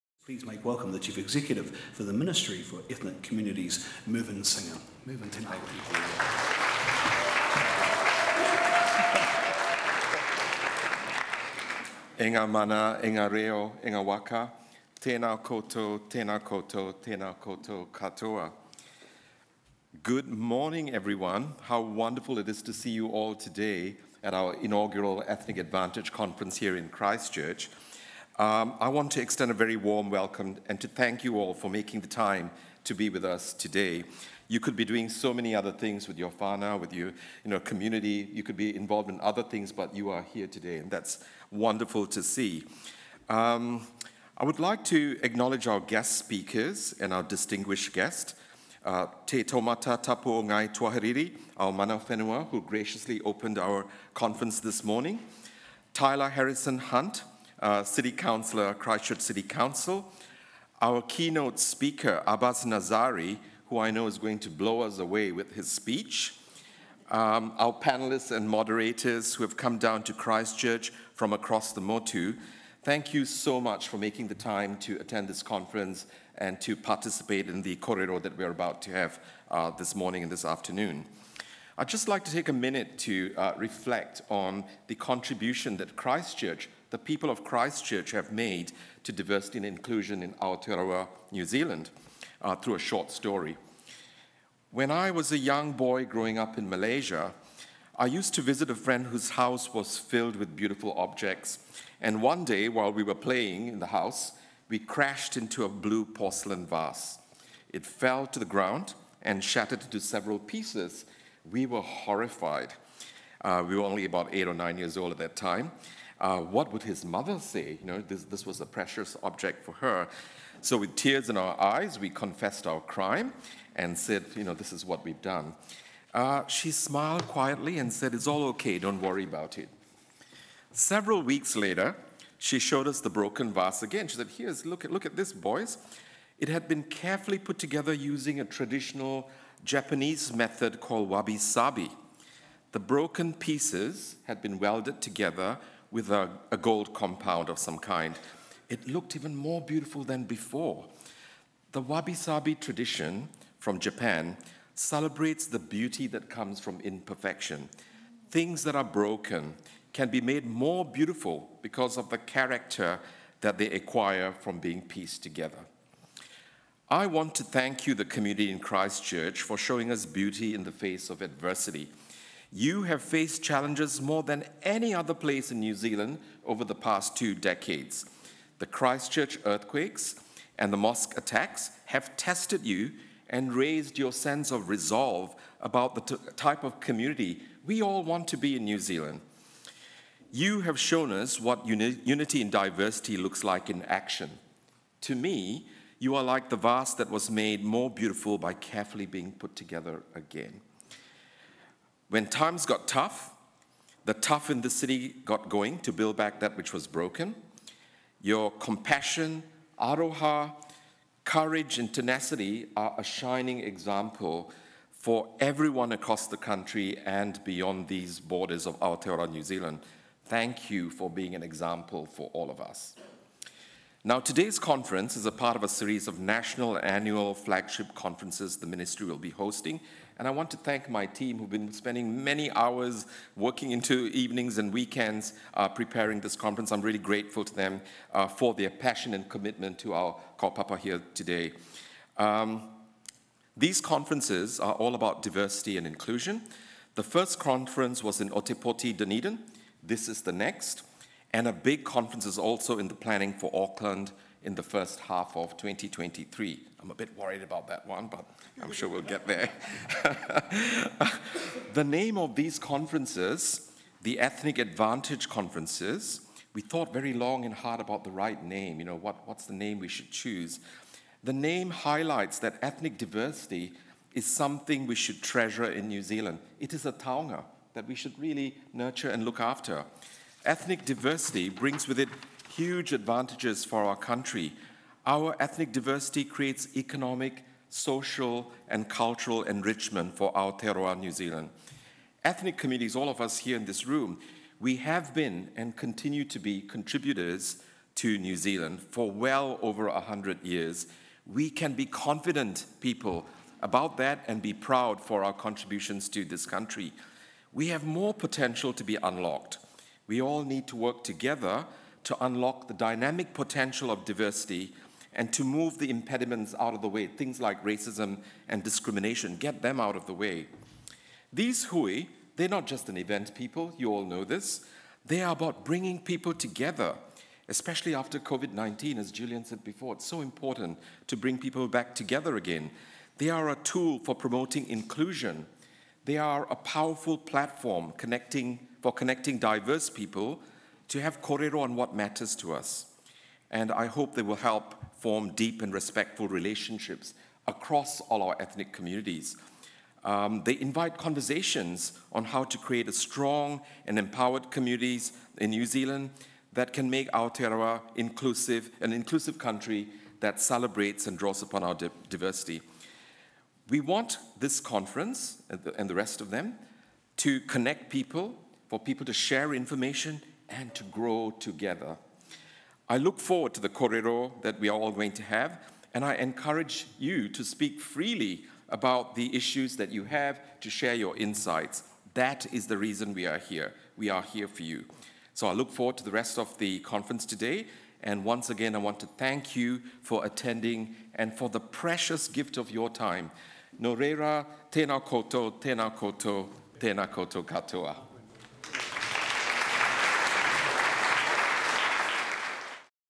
Christchurch - Opening remarks Mervin Singham
Mervin Singham, Chief Executive for the Ministry for Ethnic Communities, presents his opening remarks at the Ministry’s Ethnic Advantage conference in Christchurch.